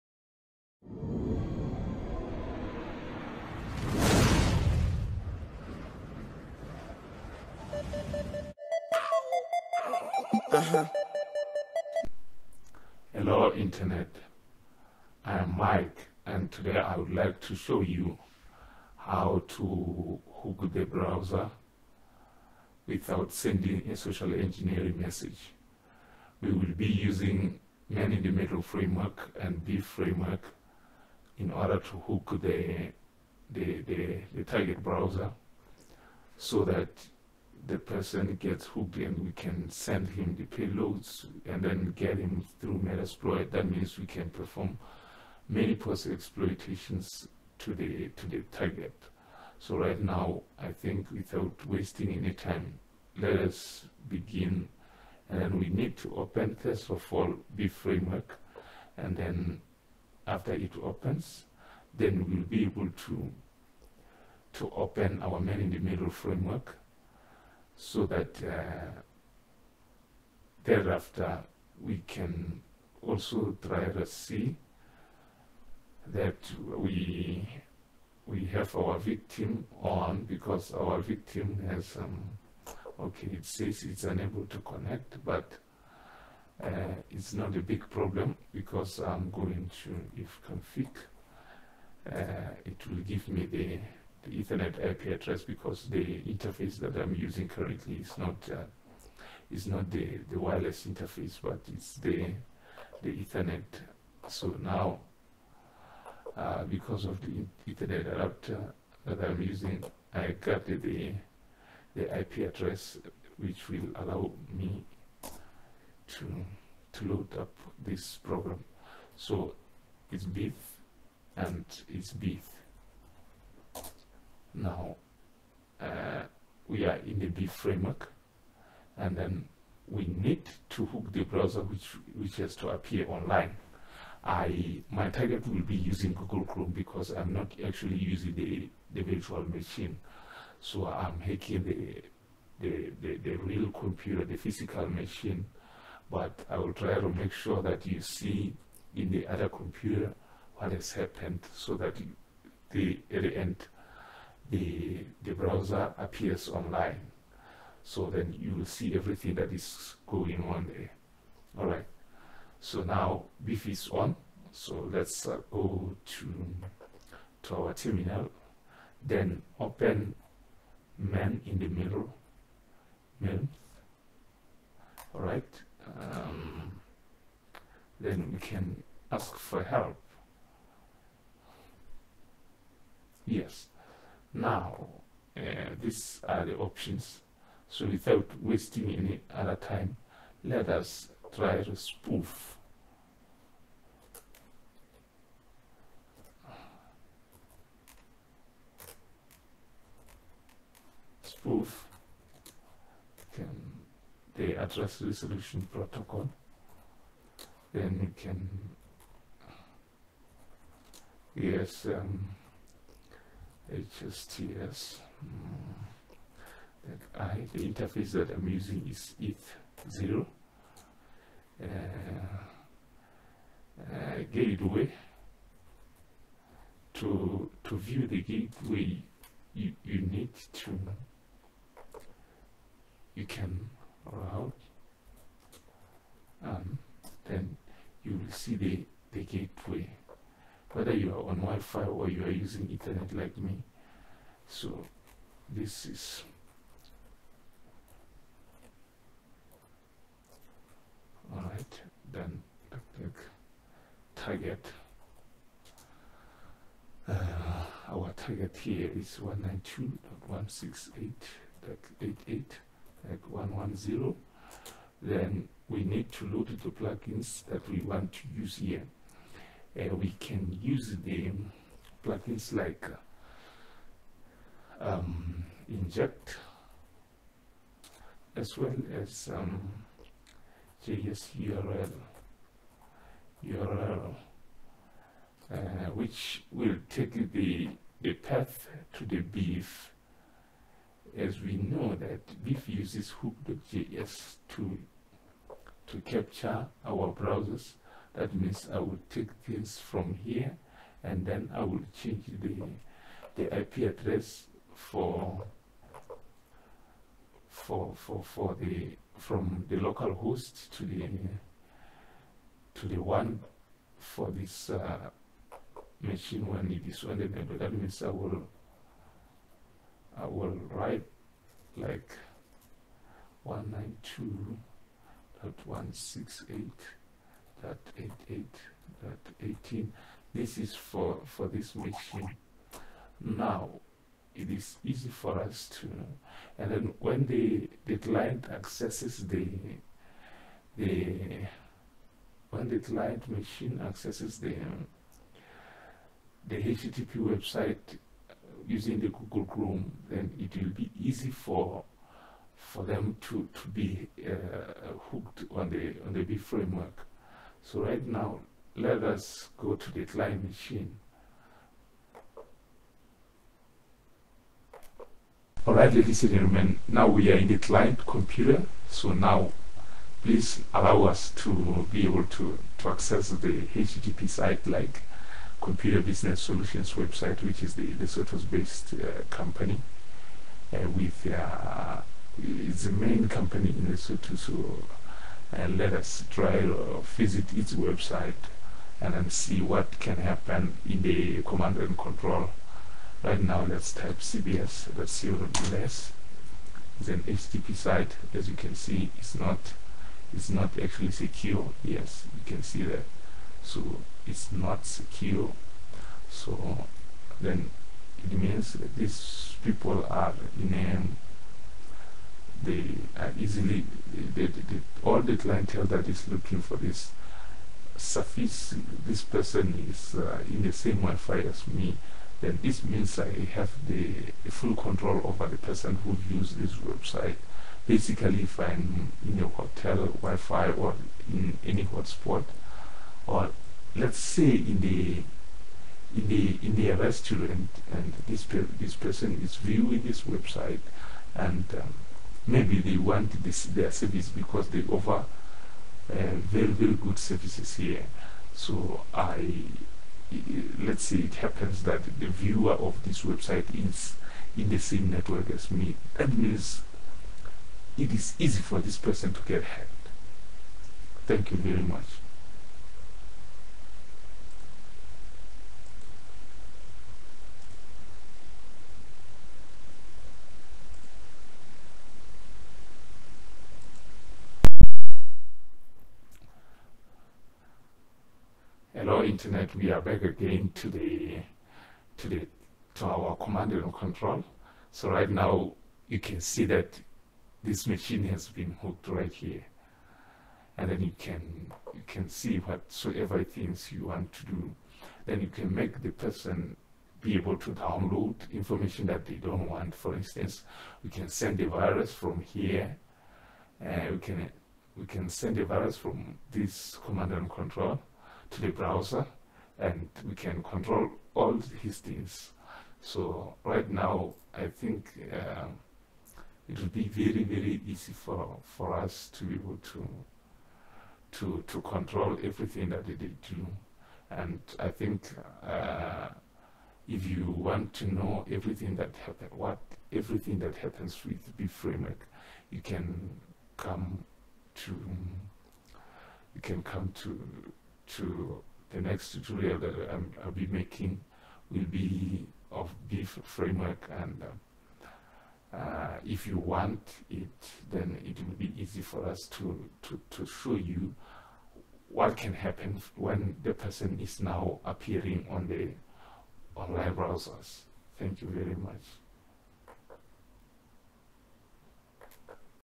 how to exploit browser without sending a social engineering messages with Beef and mitmf
MAN IN THE MIDDLE FRAMEWORK IS THE powerful framework that can allow you intercept the network traffic, it also has mutiple plugins that allow javascript injection to the browser. for we all know that JS is easily executed in the browser. we have hooked the browser using MITMF
Multiple thaNKS TO MOVAVI DEVELOPMENT TEAM.